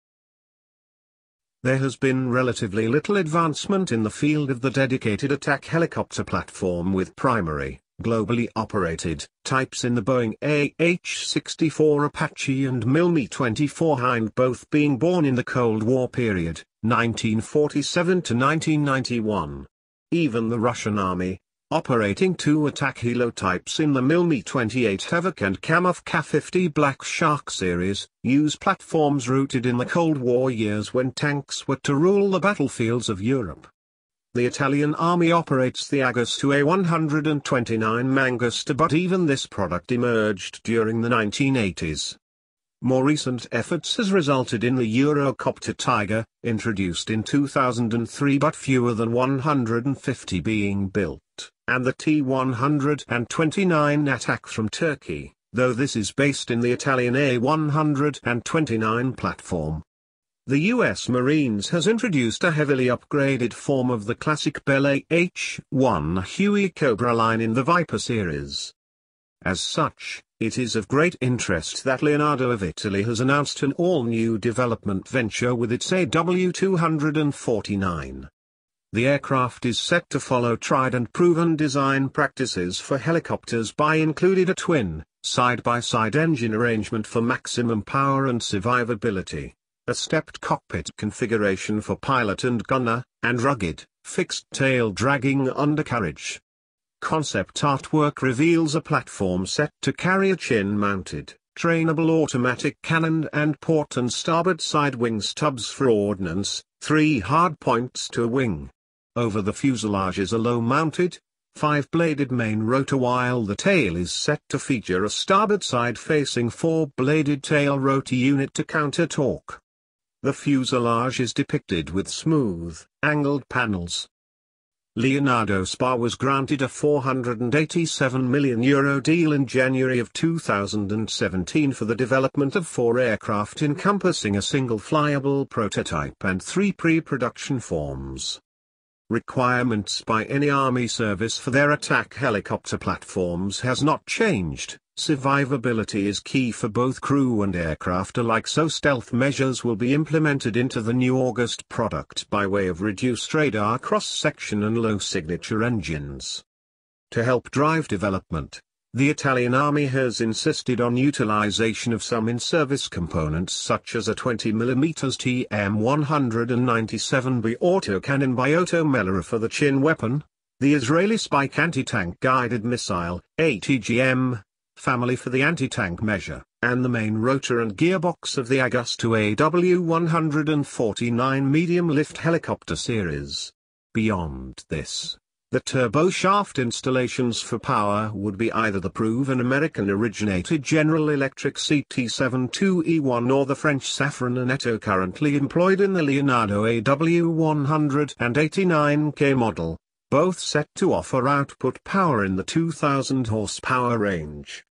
Leonardo AW249 Attack Helicopter (Italy)
Details of the proposed AW249 dedicated attack helicopter by Leonardo of Italy.